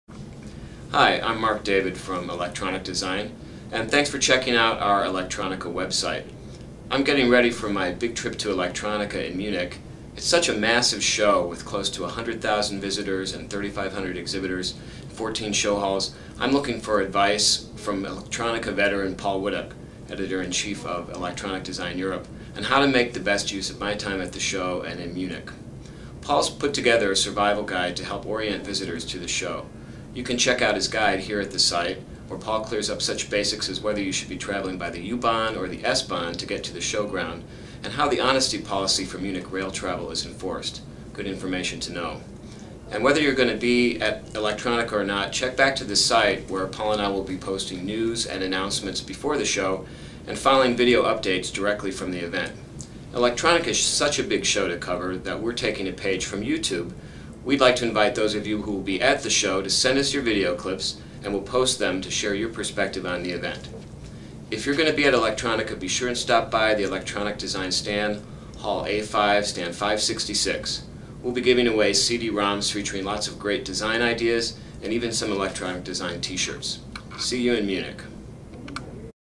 Electronic Design - Electronica 1
Mark David, editor in chief of Electronic Design before he boards the big old plane to Germany